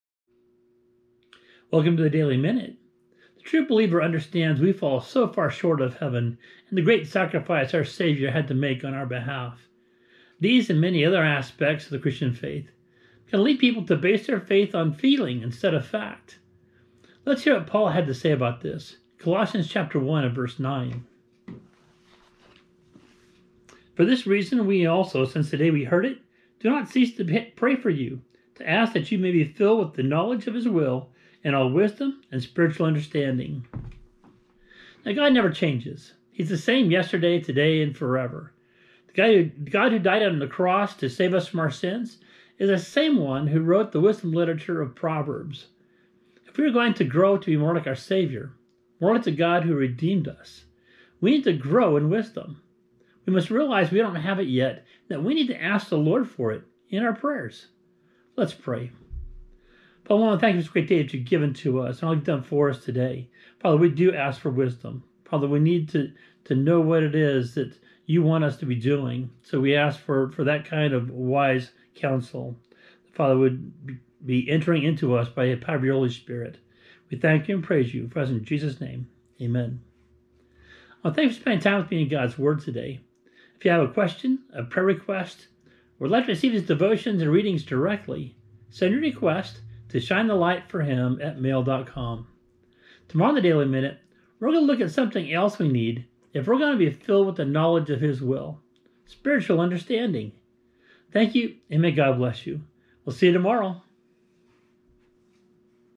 The Daily Minute for 3/22/23
Praying for Spiritual Blessings - Colossians 1:9c (NKJV)